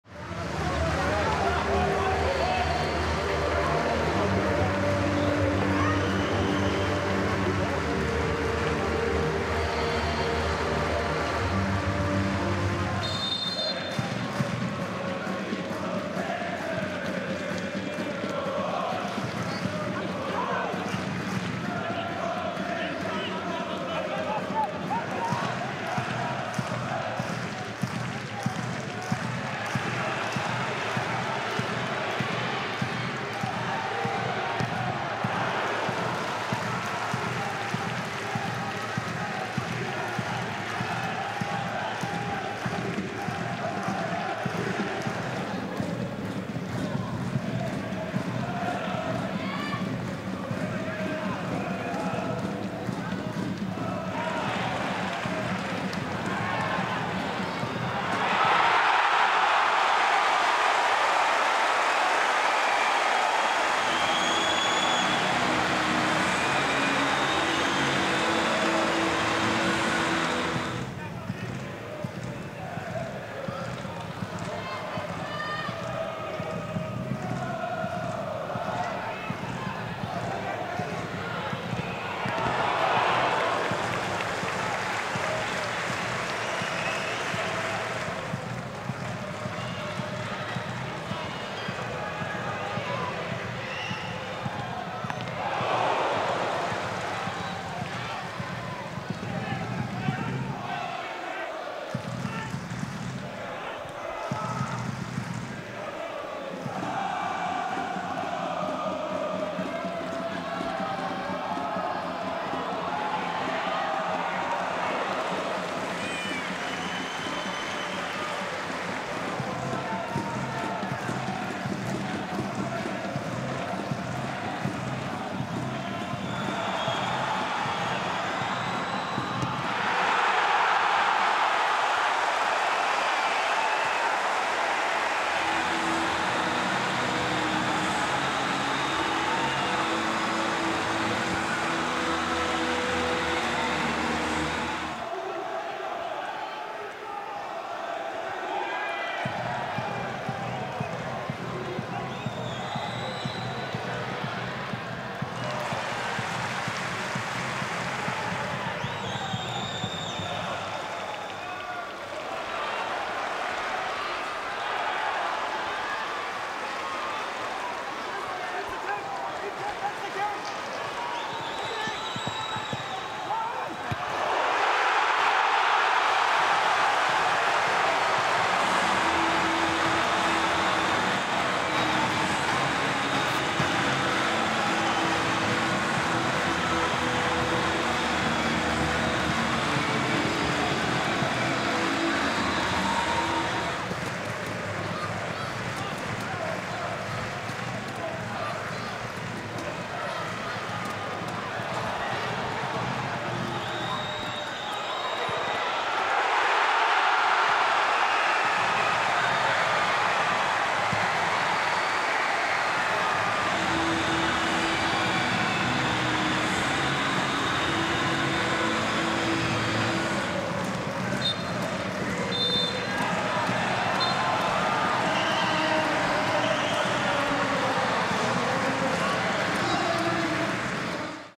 Sarpsborg 08 - Tromsø 4-0
Sarpsborg 08 smadret Tromsø 4-0 på Stadion! Scoringer: Maigaard, Opseth, Skålevik, Sandberg.